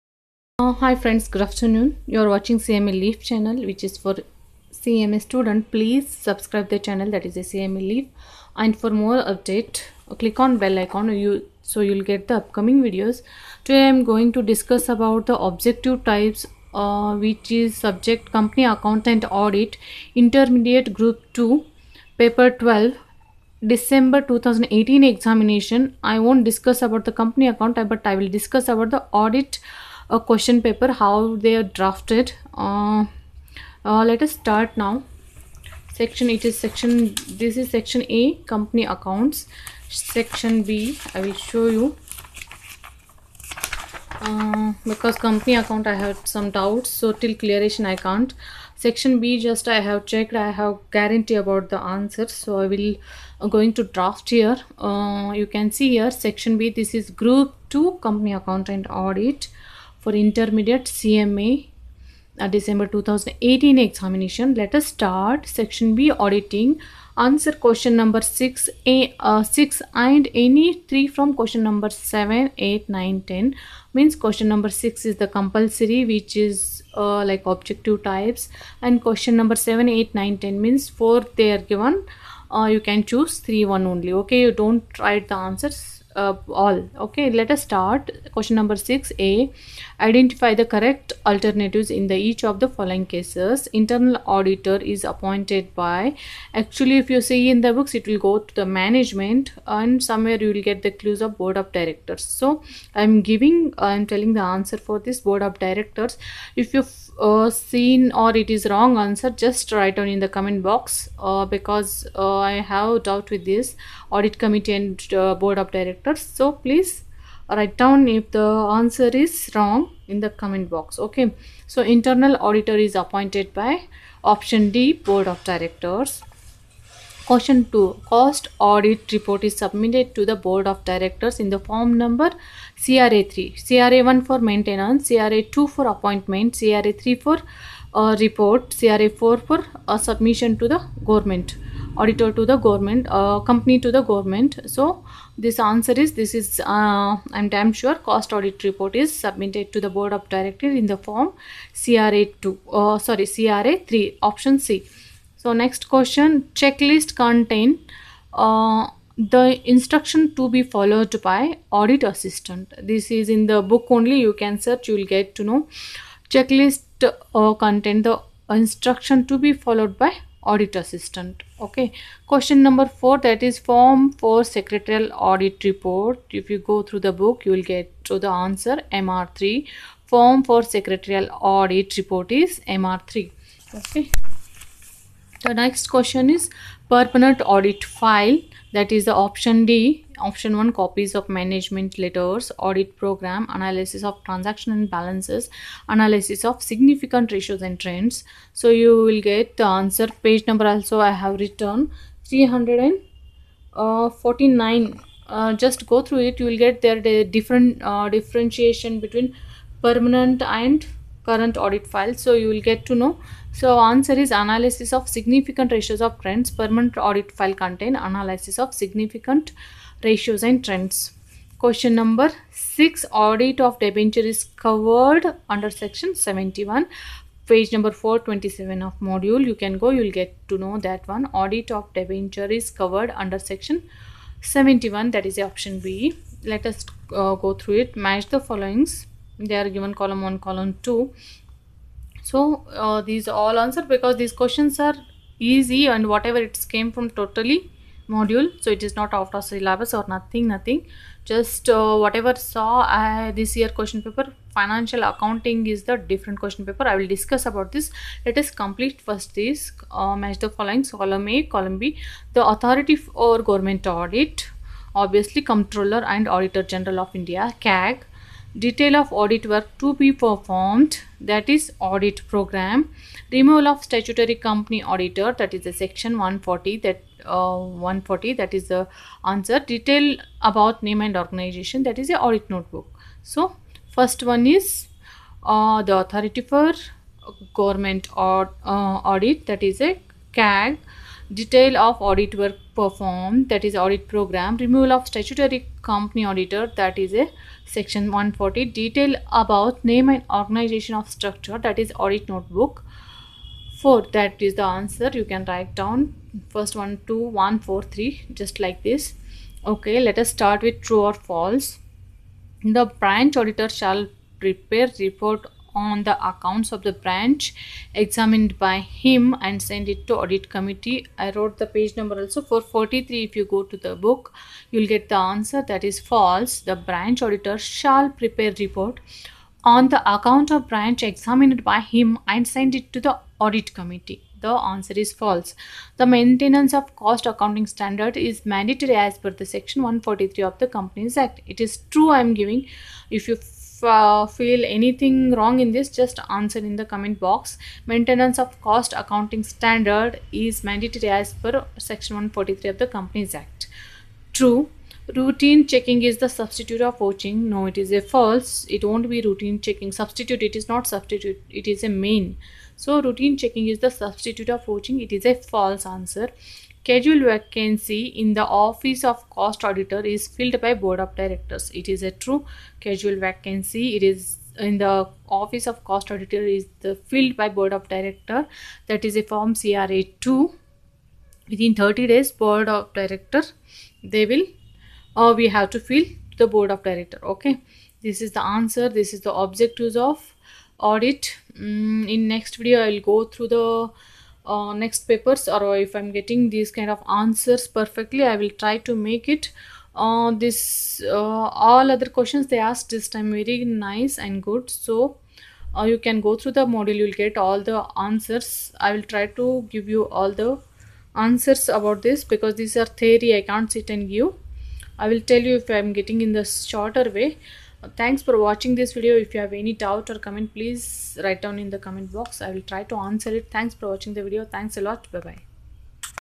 Icmai December 2018 question paper solution for company accounts and audit
Icmai December 2018 Question paper  solution for company accounts and audit, objective questions

May be you like this

books purchase online link:
Flipkart:
cma foundation books
cma inter books
cma final books